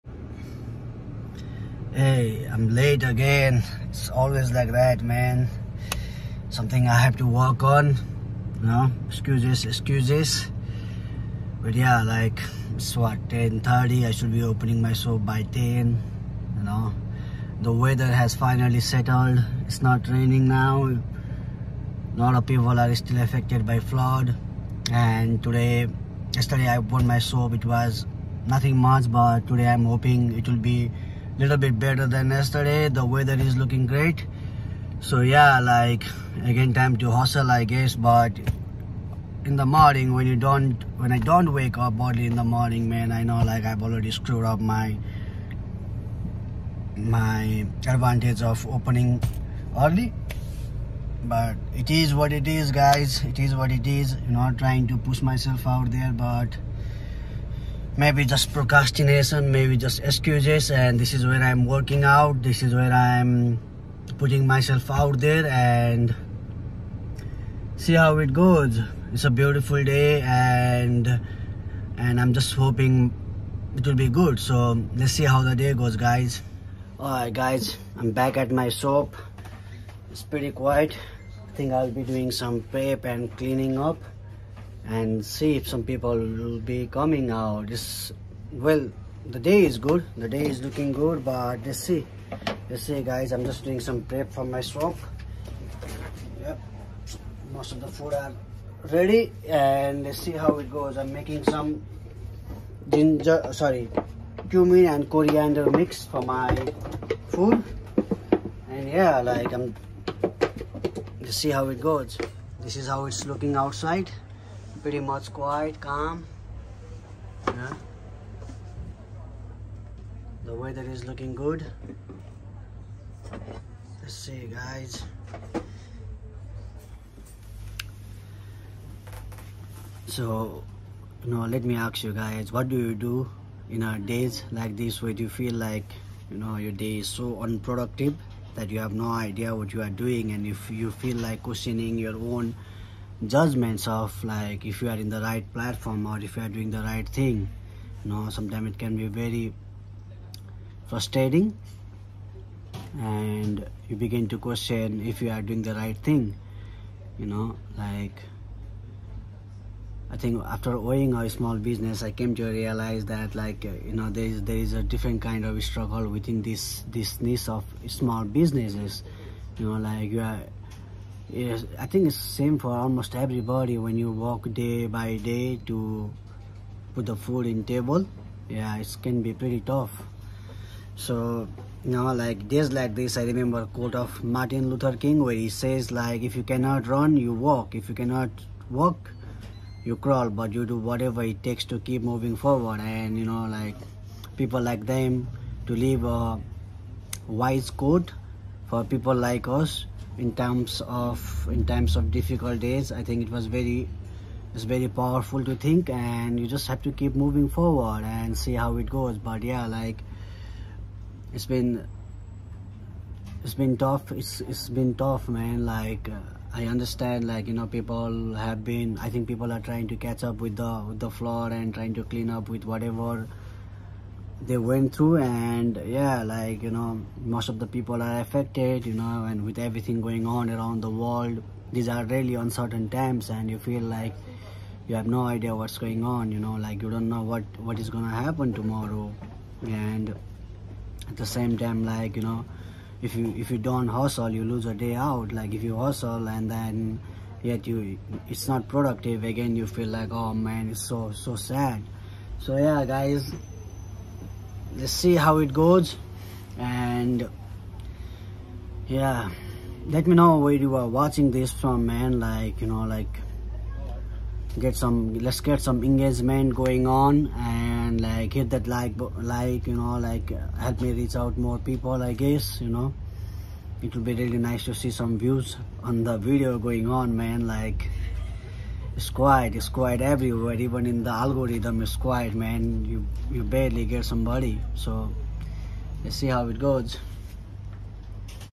Excuses excuses excuses
In this video I talk about my excuses and problems of small business owner. And also quote martin lurther king for little motivation.